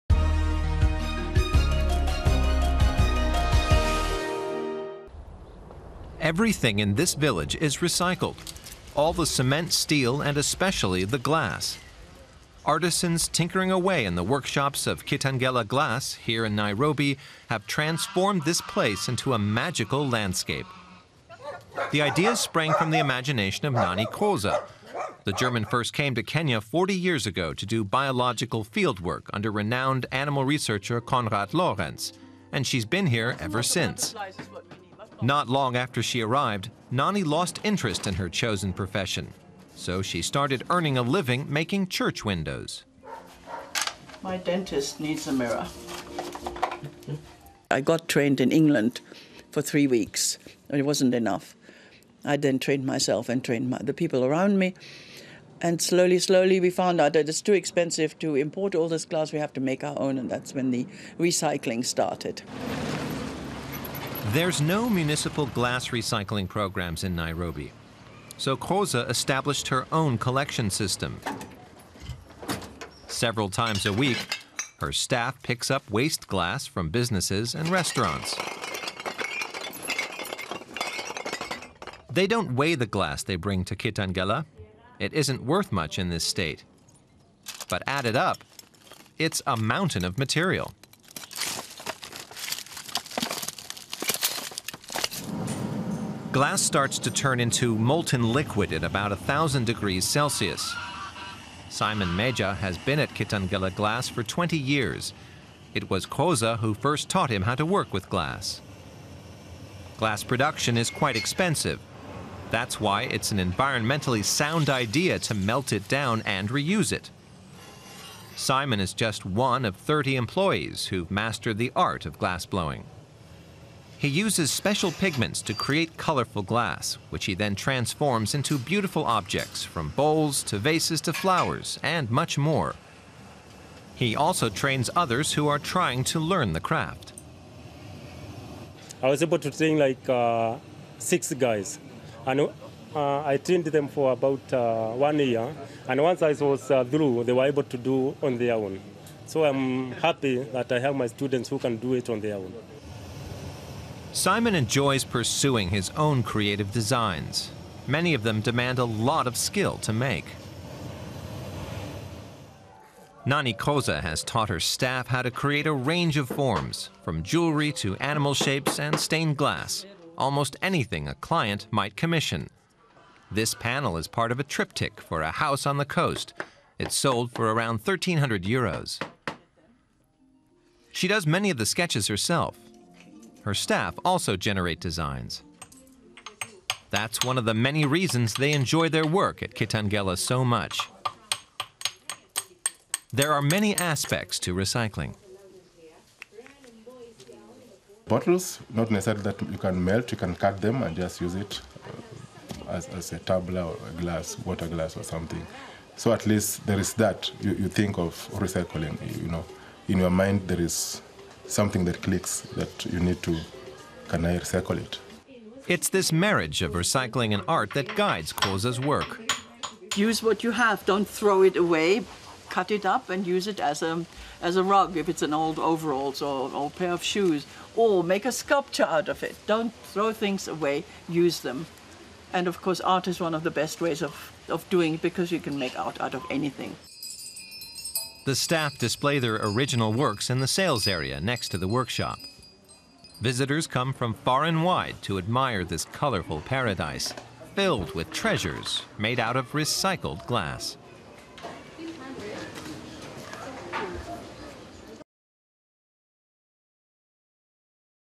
Art of glass | Eco-at-Africa
One person's trash is another's treasure. German artist Nani Groze makes stunning artwork from recycled glass in Kenya.